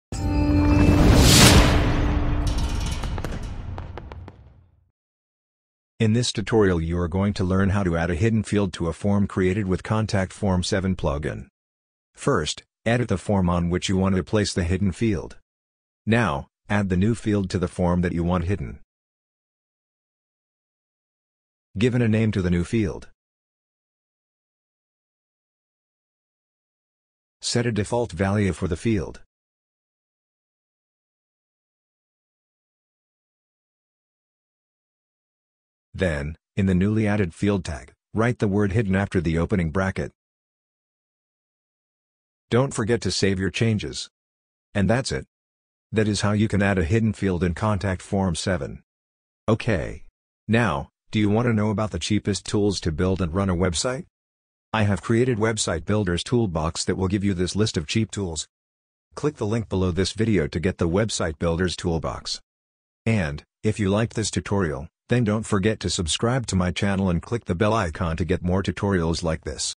Add A Hidden Field In Contact Form 7 Tutorial
This video will show you the exact steps to add a hidden field in a form created with Contact Form 7 plugin.